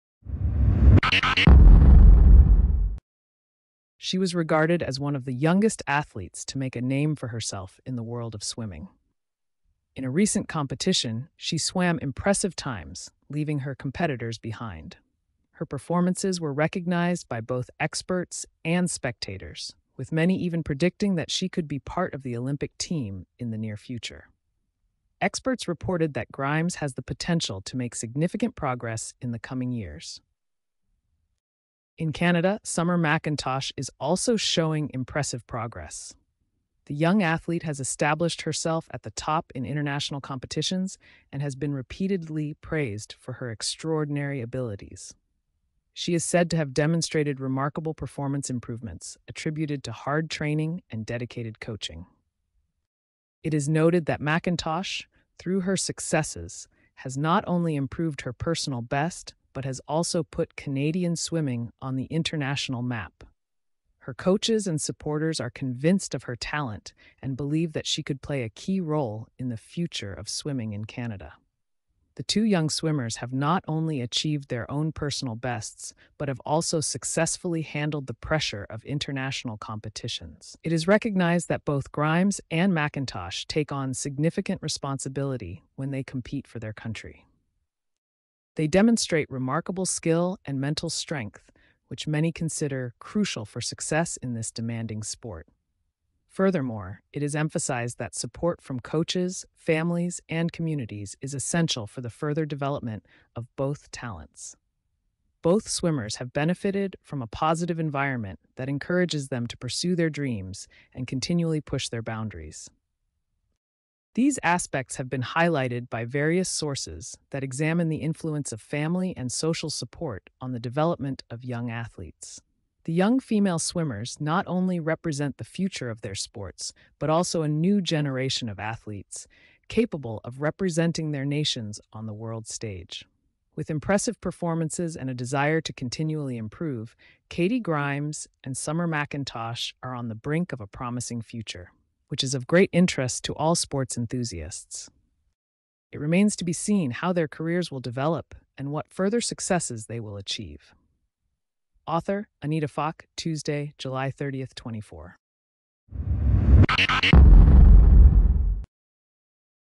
McIntosh and Grimes Canadian teen excels in swimming (Podcast)
The achievements of young female swimmers attract more attention. AI-generated content